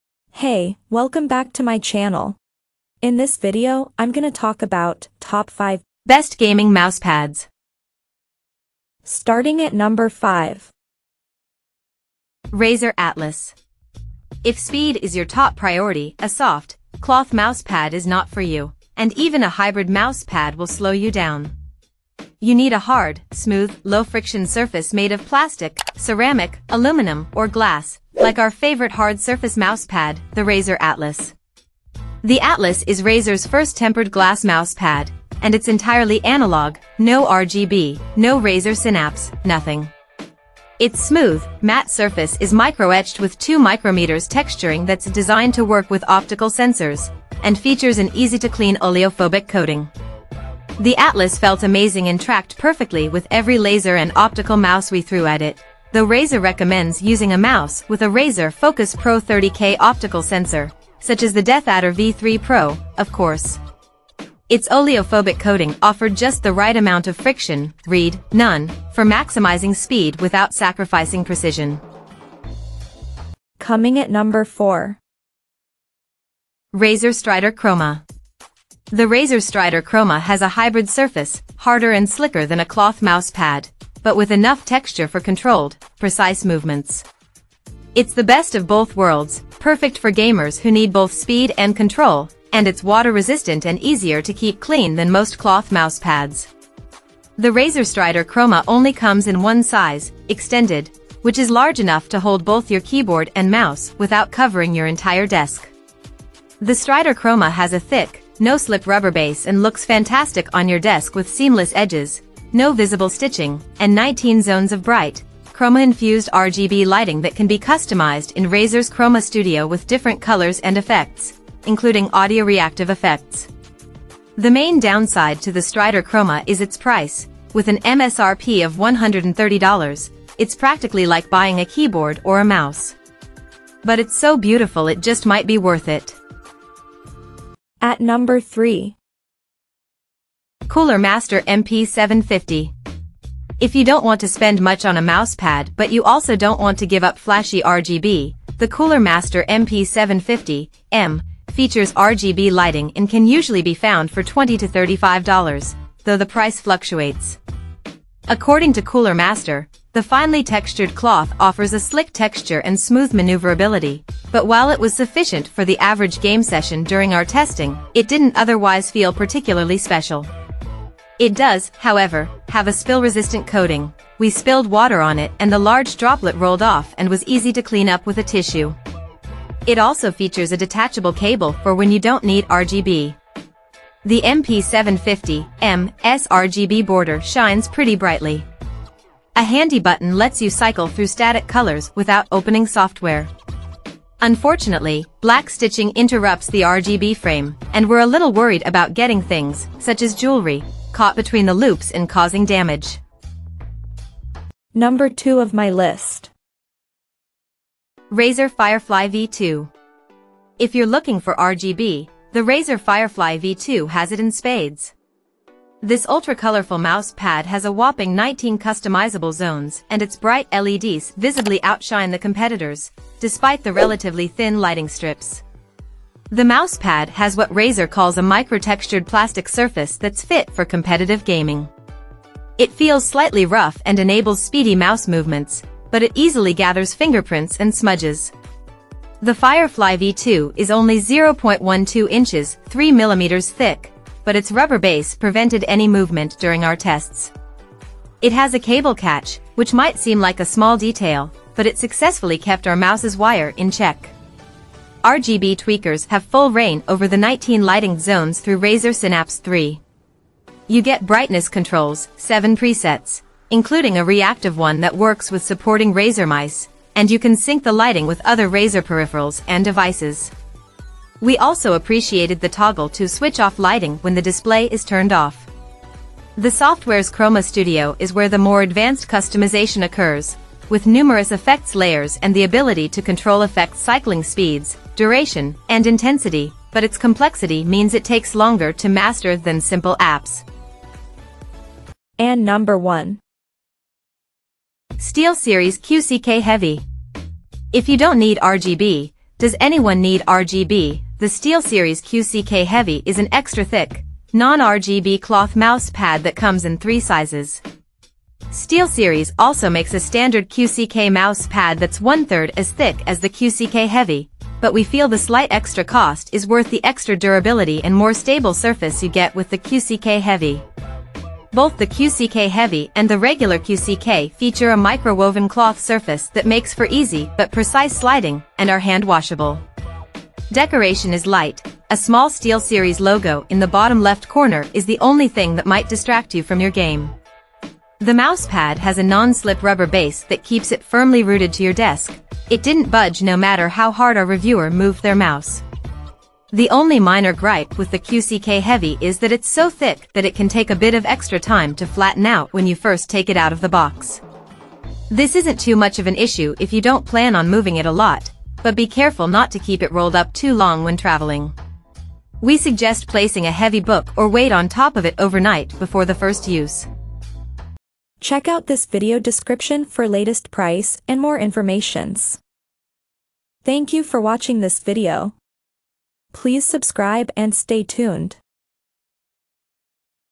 Top 5 Gaming Mouse Pads in 2024
Here is the Updated Prices of the Best Gaming Mouse Pads in 2023!

Things that we mentioned in this video:
1. Steelseries QCK Heavy - B000V7ARAU
2. Razer Firefly V2 - B07ZDWHL4M
3. Cooler Master MP750 - B07JFPBS66
4. Razer Strider Chroma - B0B75YMDMC
5. Razer Atlas - B0BWLZ65D7